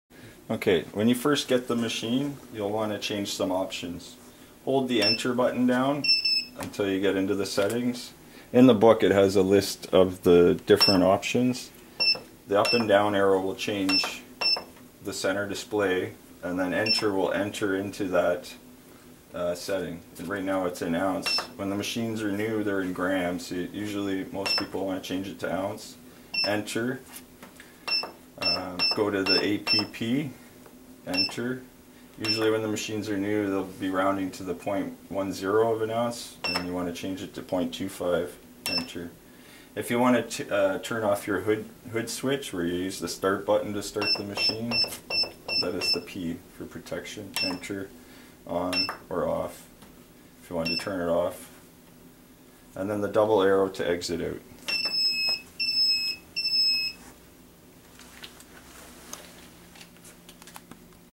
MAJOR Wheel Balancer CBL-3560 | Setting Up
Major Wheel Balancer CBL-3560, A-D Data Entry Automatically. Quicker and easier to balance a wheel more accurately.